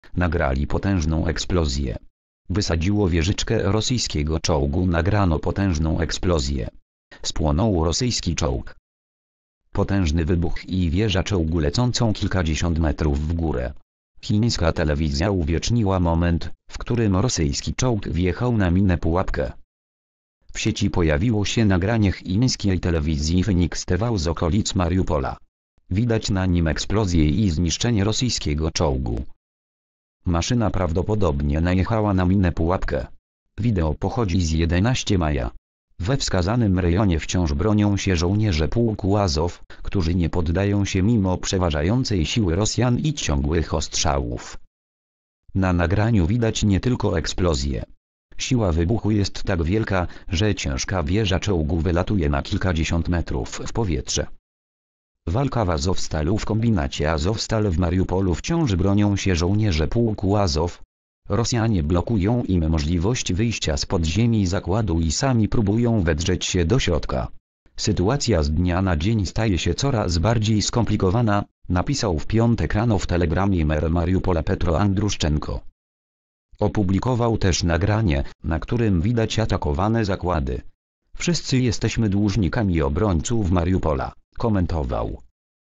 Nagrali potężną eksplozję. Wysadziło wieżyczkę rosyjskiego czołgu.
Nagrano potężną eksplozję. Spłonął rosyjski czołg.

  (fot. Twitter.com/ - screen shots video)